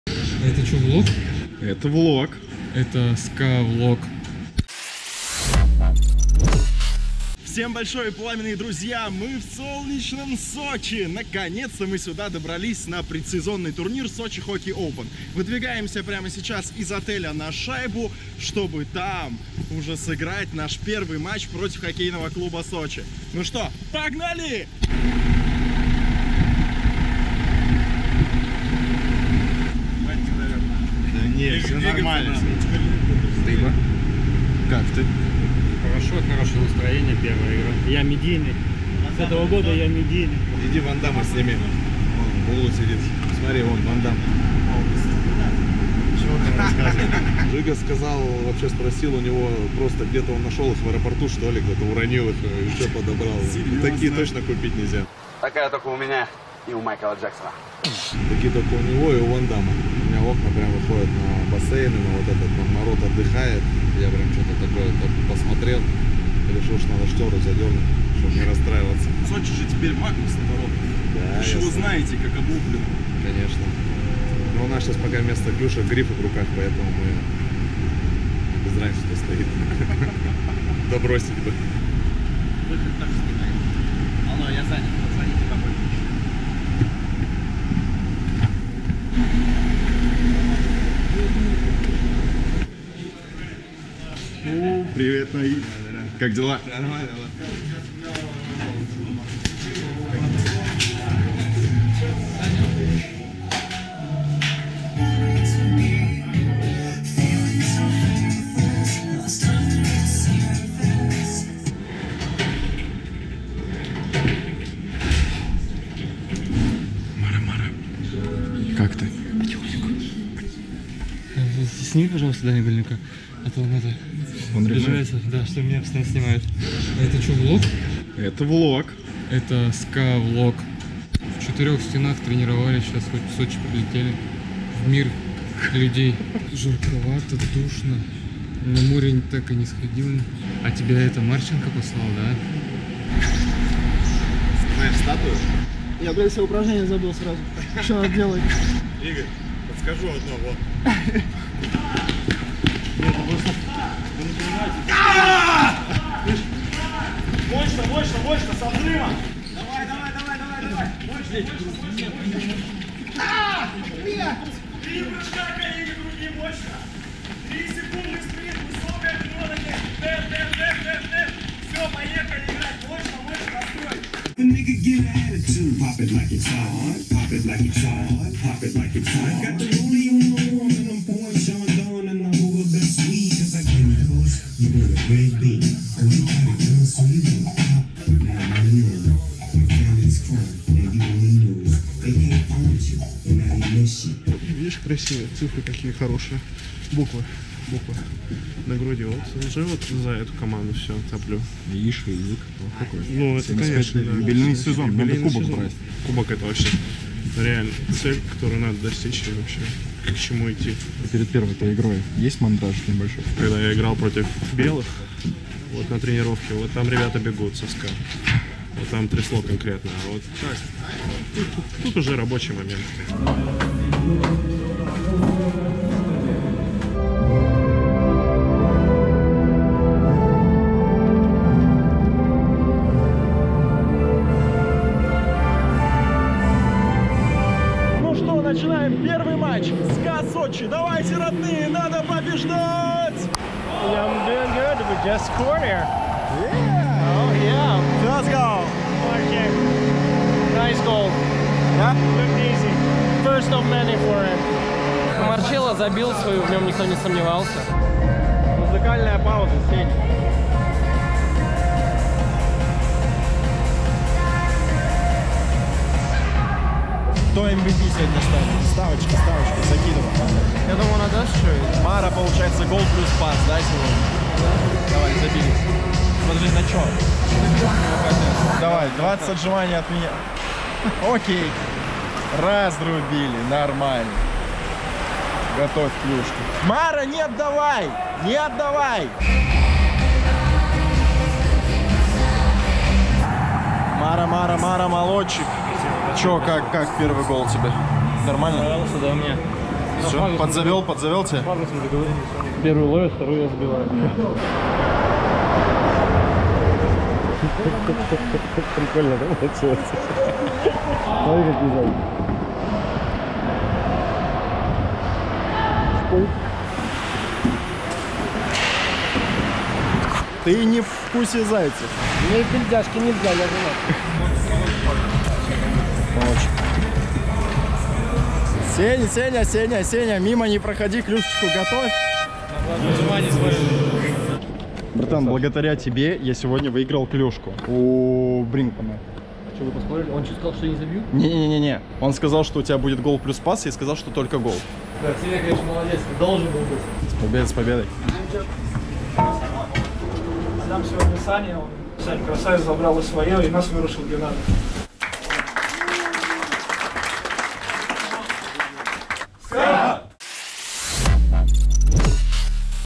SKAVlog. СОЧИ. ЖАРА. ПЕРВАЯ ПОБЕДА
Температура в Сочи зашкаливает🌴 И мы не о погоде, а о нашем SKAVlog, который получился просто раскаленным. Такой только у нас и у Ван Дамма!
Супер-очки Кузьменко, страдания по бассейну, танцевальная пауза, спор на клюшку и много-много хоккея. Ныряем в автобус, в раздевалку - и на лед!